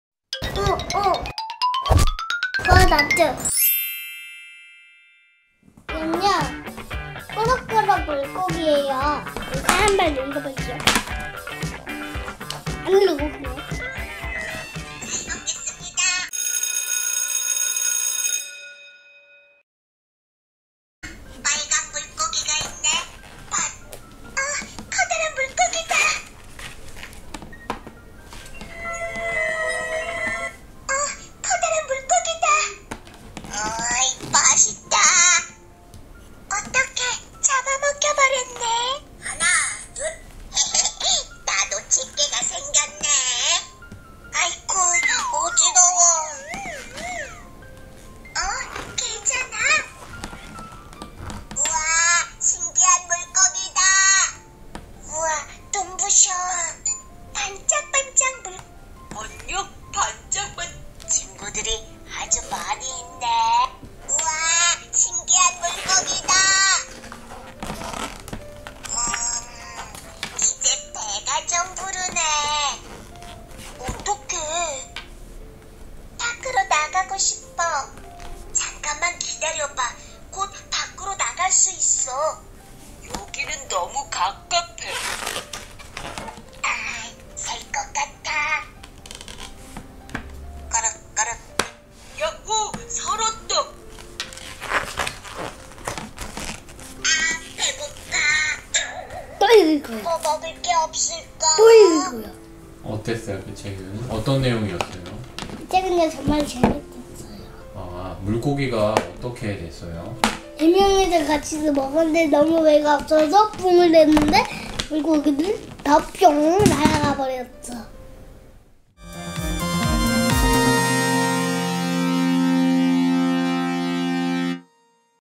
세이펜 독서시간. 꼬륵꼬륵 물고기 [아람출판사]
안녕하세요, 우우브라더스입니다.
아람출판사의 바나나 창작동화 중
"꼬륵꼬륵 물고기"를 읽어보았어요!
중간 중간 구멍이 나 있는 물고기 배 속으로,
어떤 것들이 들어갈까요?
다같이 읽어보아요^^